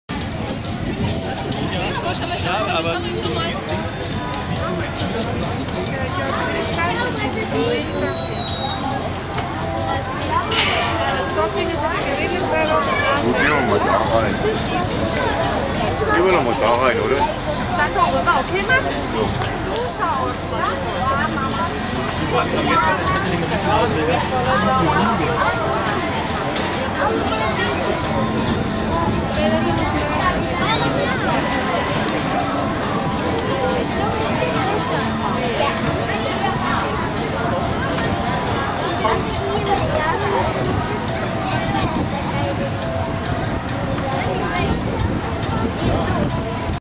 Legoland Günzburg: Kuka Roboter verstoßen symbolisch gegen die Kuka-Richtlinien
Test des Panasonic Camcorders HC-X909 im Legoland Günzburg: Hier wird mit Lego-Männchen gezeigt, was man mit Kuka-Robotern eigentlich auf keinen Fall machen darf

Testbericht war einmal auf: neuerdings. com/2012/09/10/panasonic-hd-camcorder-hc-x909-test-1/

Test of the Panasonic camcorder HC-X909 in the Legoland Günzburg: Here you can see with Lego men what you shouldn't do with Kuka robots.